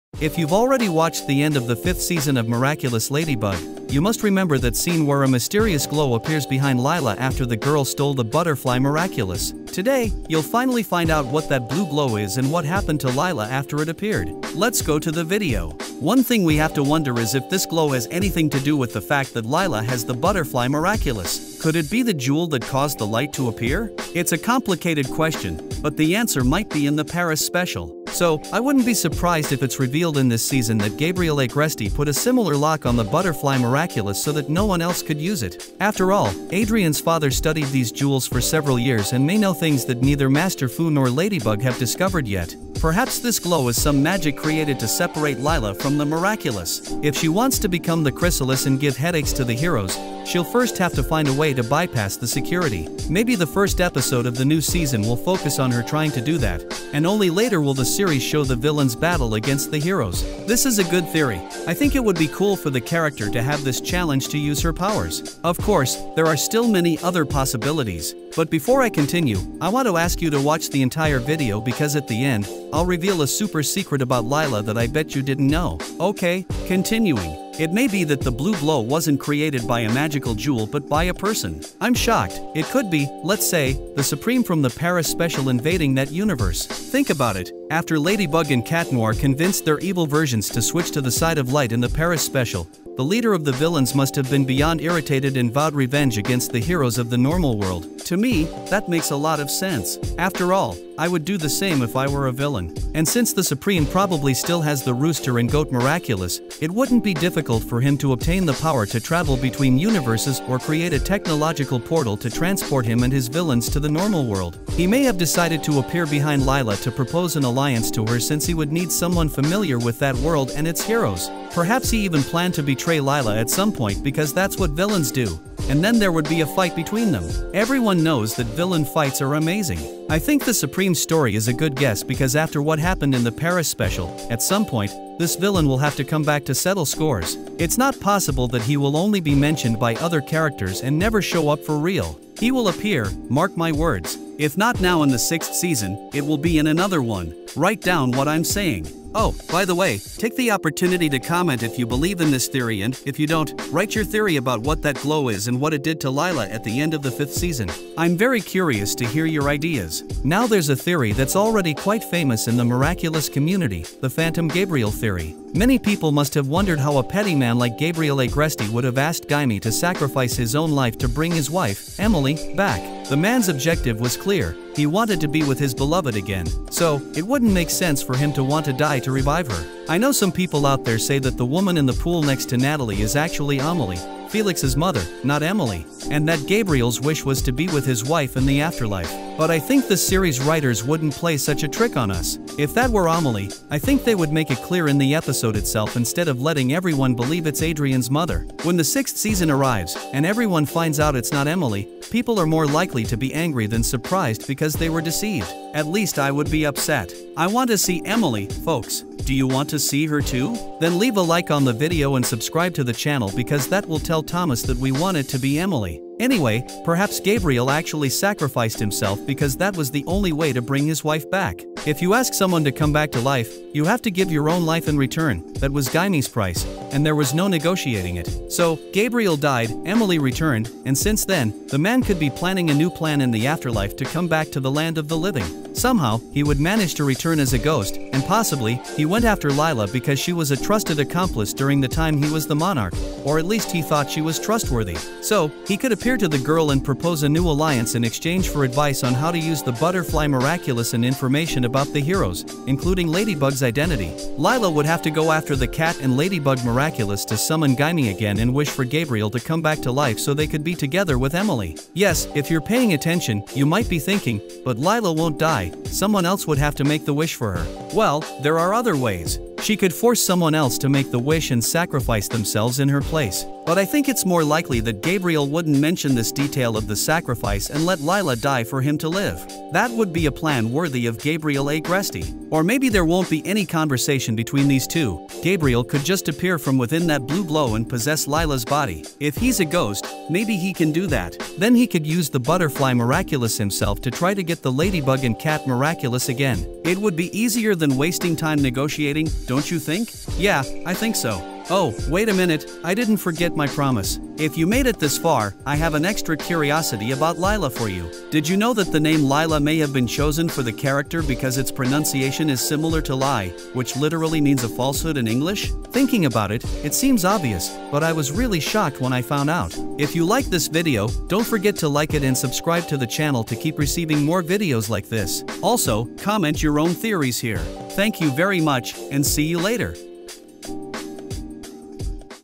WHAT HAPPENED TO LILA AT THE END OF SEASON 5 OF MIRACULOUS LADYBUG??
If you've already watched the end of the fifth season of Miraculous Ladybug, you must remember that scene where a mysterious glow appears behind Lila after the girl stole the butterfly Miraculous. Today, you'll finally find out what that blue glow is and what happened to Lila after it appeared. Let's go to the video.